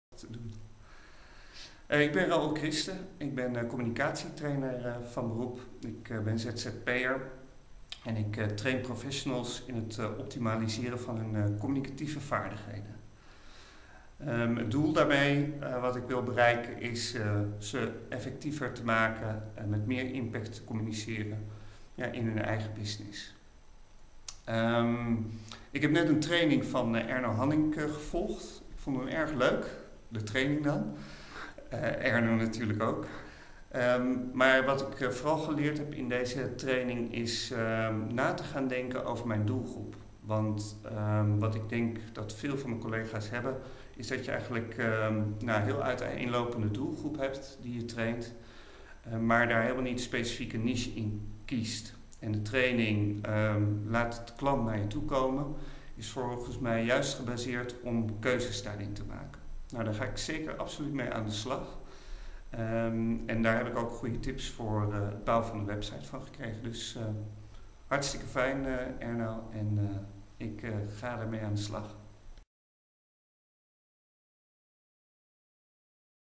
Raoul Christen video recensie workshop Laat de klant naar jou komen
Raoul Christen vertelt wat hij doet en wat hij vond van de workshop Laat de klant naar jou komen versie 2 van de video met andere kader